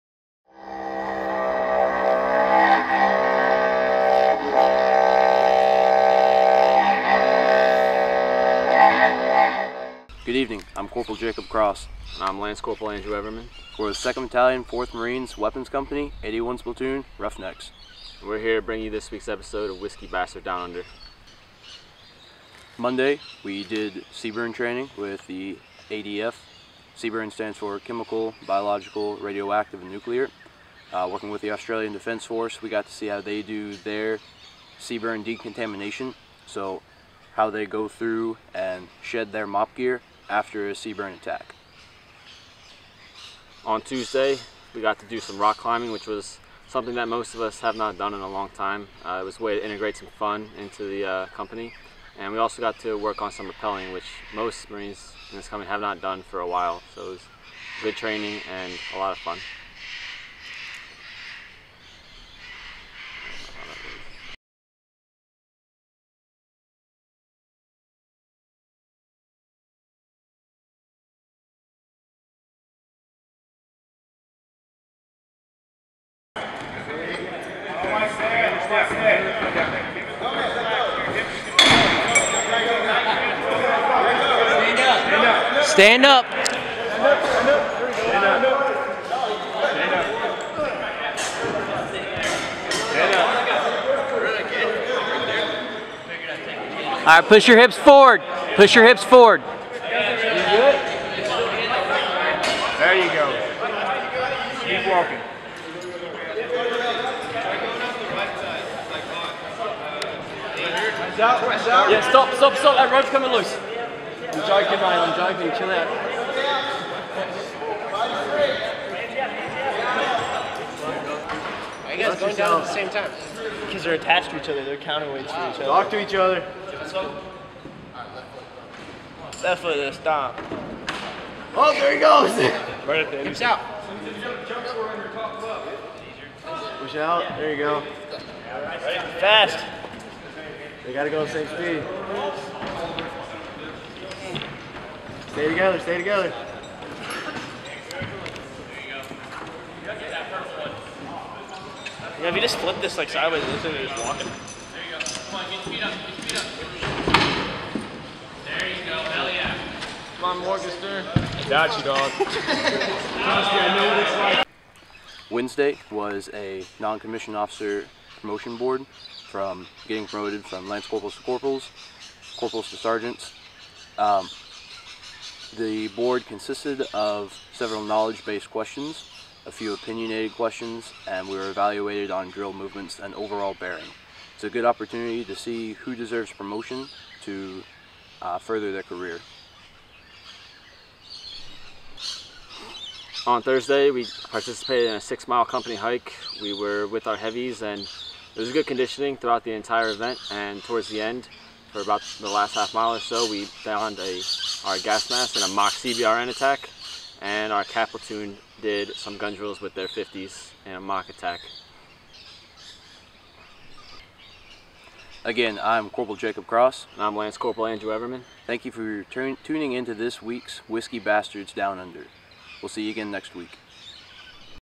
Whiskey Bastards Down Under #2
This week Lcpl. Everman and Cpl. Cross from Roughneck take you through Weapons Company's week.